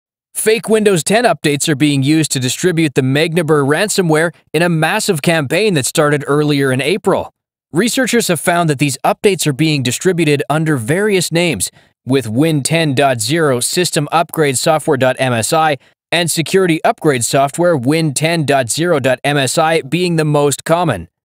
Fake Windows 10 Updates Being Used to Distribute Magniber Ransomware
Fake Windows 10 updates are being used to distribute the Magniber ransomware in a massive campaign that started earlier in April. These updates are distributed under various names, with Win10.0_System_Upgrade_Software.msi and Security_Upgrade_Software_Win10.0.msi being the most common.